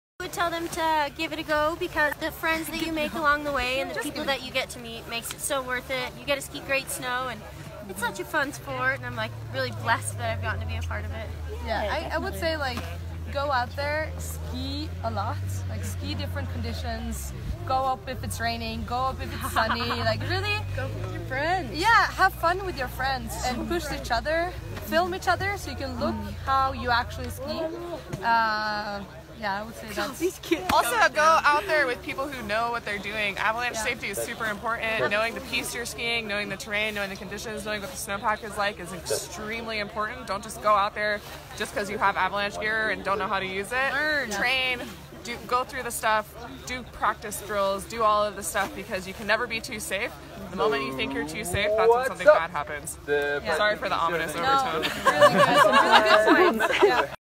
How to get into Freeriding, with Hedvig Wessel
Hedvig Wessel, women's ski winner at the Fieberbrunn round of the FreerideWorldTour in Austria, along with snowboarder, Erika Vikander and skier, Jacqueline Pollard give some sound advice for anyone wanting to get into Freeriding.